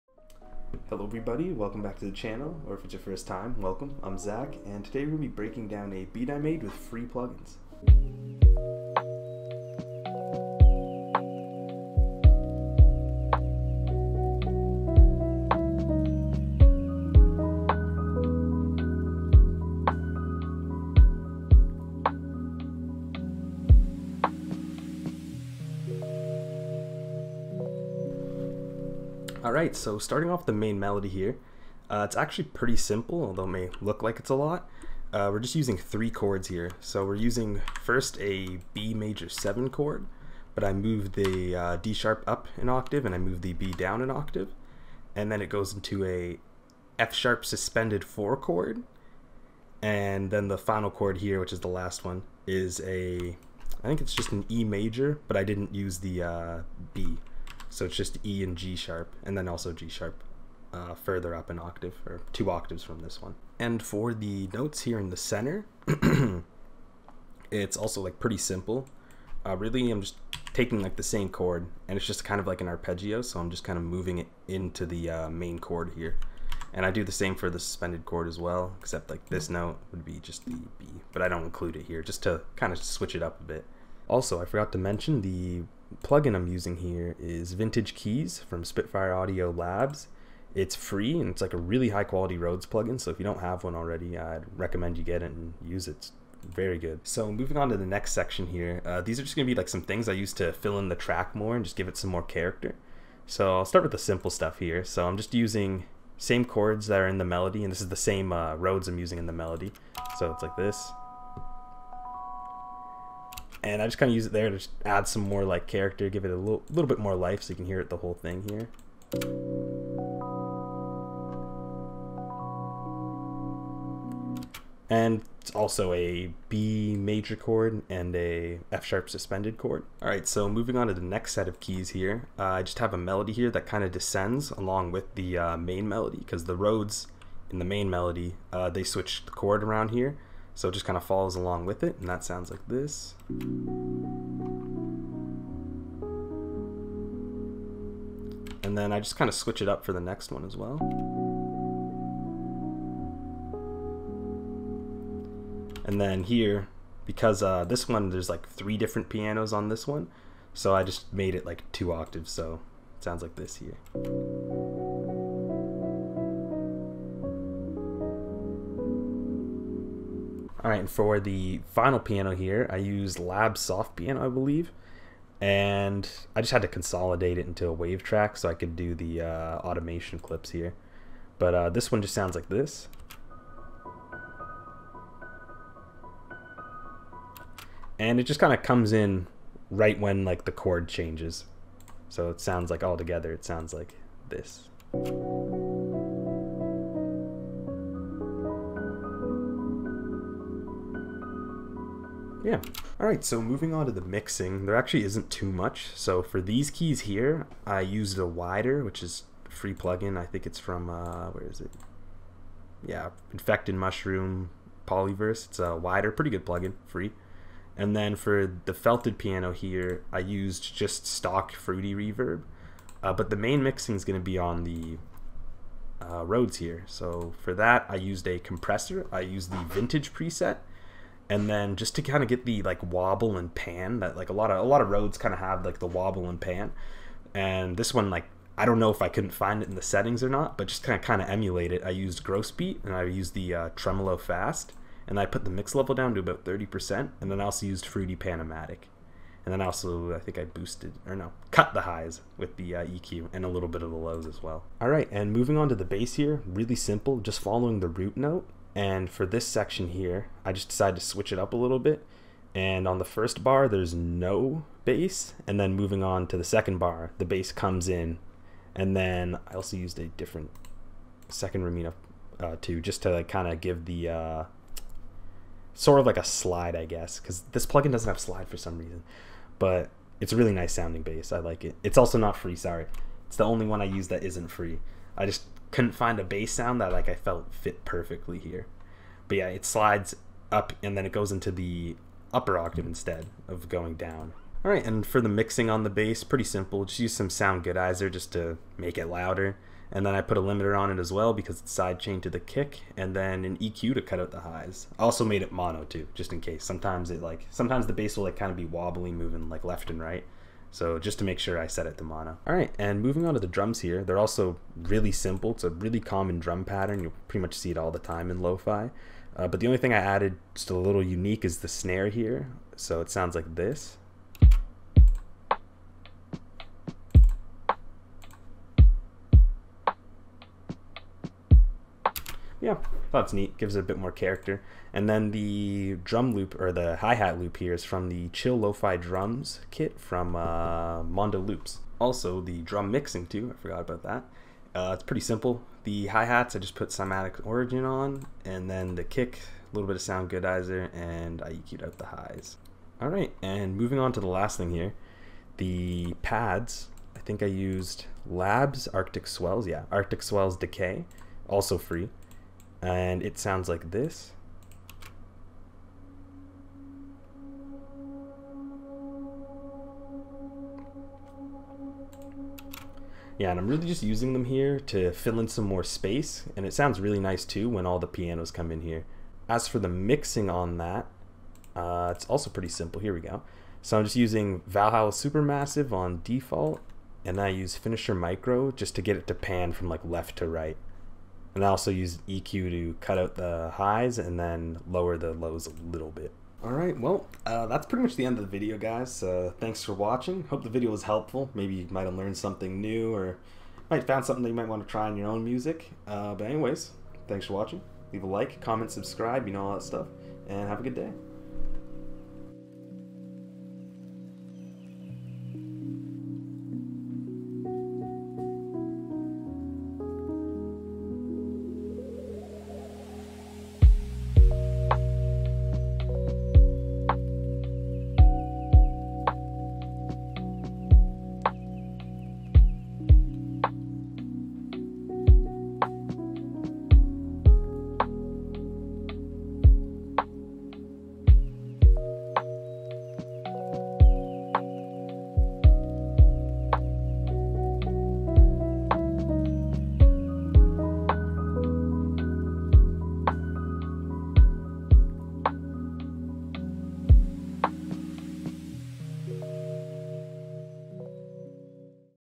Dreamy Vibes ~ How to make a Lofi Beat Using Free Plugins FL Studio 21
Really just wanted to showcase the new LABS instrument. It's a really good Rhodes plugin, I wish it was around when I started.

👇 Plugins I used (in order):
LABS ( Vintage Keys)
LABS ( Tape Piano)
Labs ( Soft Piano)
LABS ( Arctic Swells)
Kee Bass ( I didn't use it here but its a good synth bass plugin)

👇 Effects Plugins:
Wider
Valhalla SuperMassive

⌛ Timestamps:
Intro/Teaser 0:00
Main Melody 0:29
Mixing Keys 3:22
Bass 4:14
Drums 5:13
Pad 5:52
Outro + Full Beat 6:36